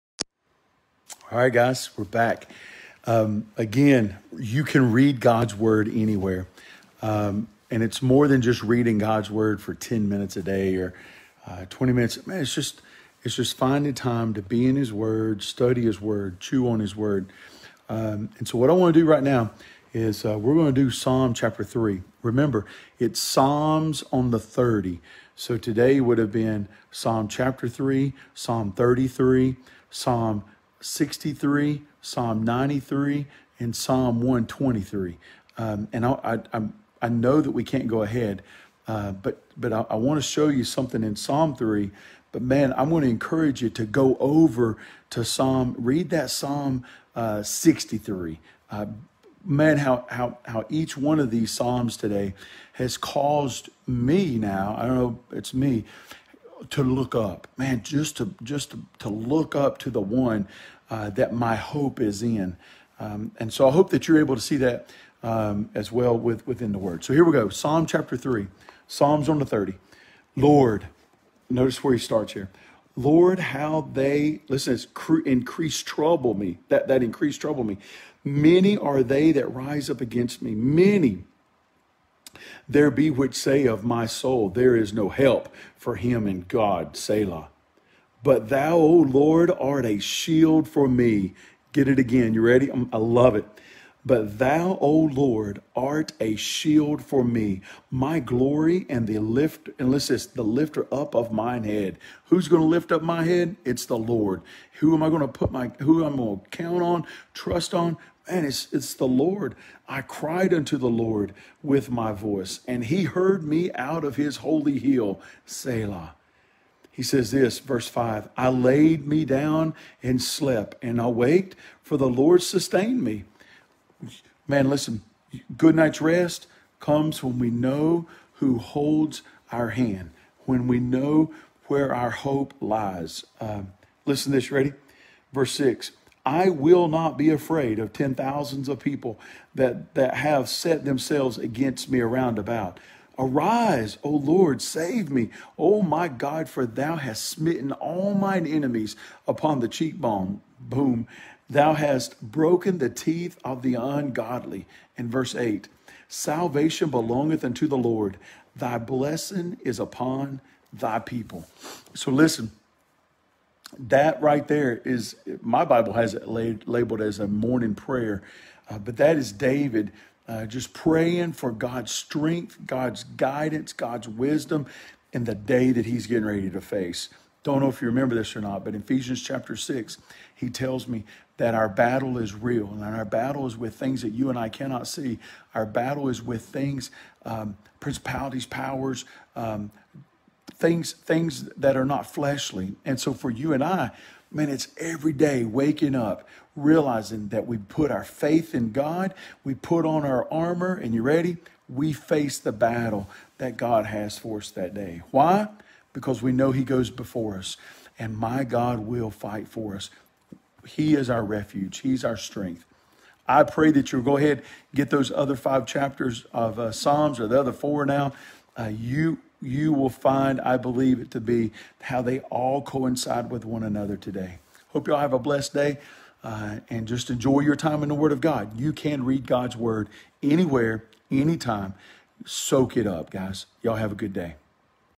5 Psalms a Day - Psalm 3
Today we are reading Psalm 3
You should read Psalm 3, 33, 63, 93 & 123
Pastor Scott Townsell
South River Baptist Church - Statesville, NC
To listen to our sermons on audio go